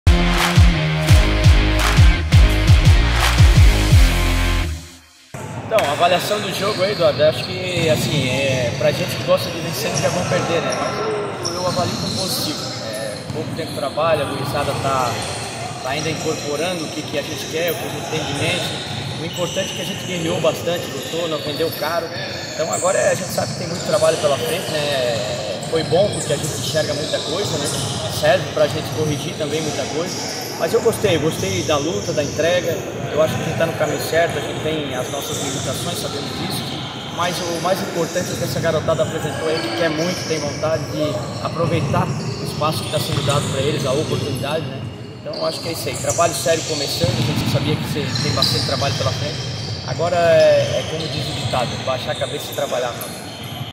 11/03/2023 | JOGO-TREINO ADFT SUB17 | Avaliação Pakito
ADFT/Tubarão Futsal Sub17, perde jogo-treino para ACR/Criciúma

No sábado (11), à ADFT/Tubarão Futsal recebeu à ACR(Criciúma) para um jogo-treino, com um bom público formado por familiares de atletas acompanharam a partida, em quadra derrota pelo placar de 2x0.

Esse foi o primeiro teste do elenco sub17, após 20 dias desde o início da preparação, o Esporte de Primeira acompanhou o jogo-treino, e Pakito fez uma avaliação do trabalho realizado na Arena Multiuso Estener Soratto da Silva.